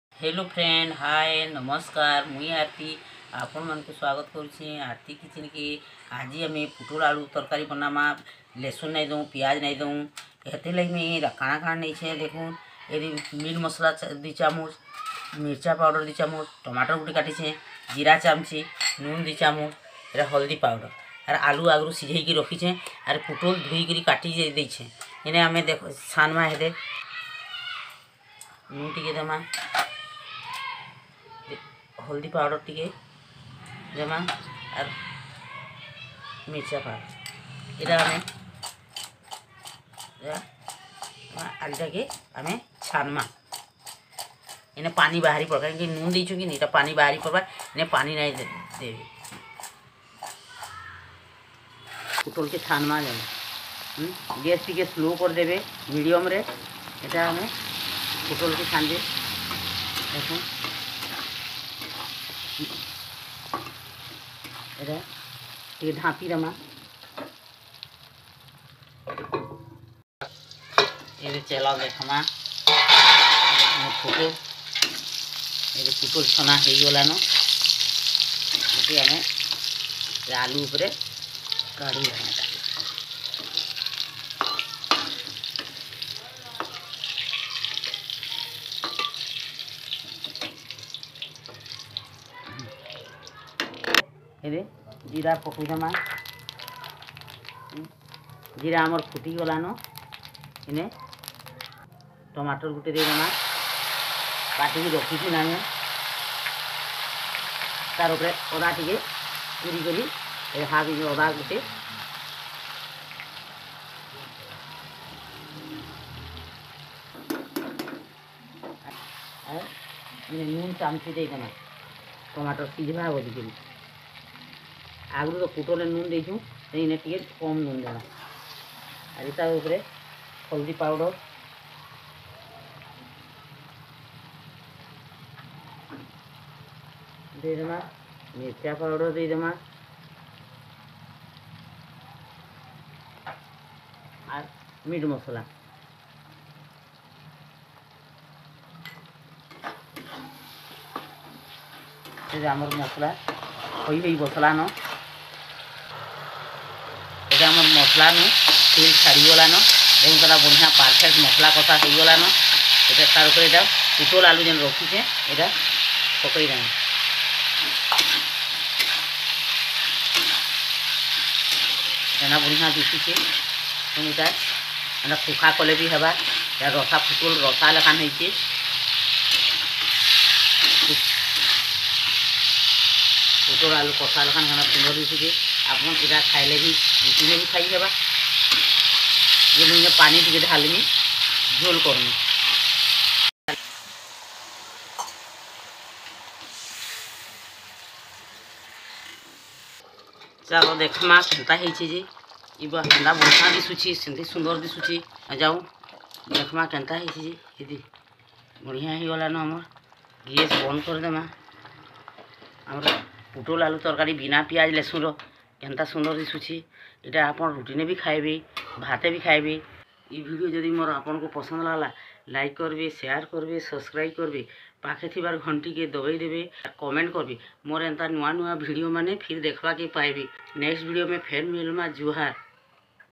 ପୁଟଲ୍ ଆଲୁ ତରକାରି || Odia Potala Alu Jhol || How to Prepair Alu Potala Tarakari || Arati KITCHEN
Hello,
        Today very special "potala alu tarakari" recipe.one of the my favorite recipe.I hope you enjoy this video. If you like my video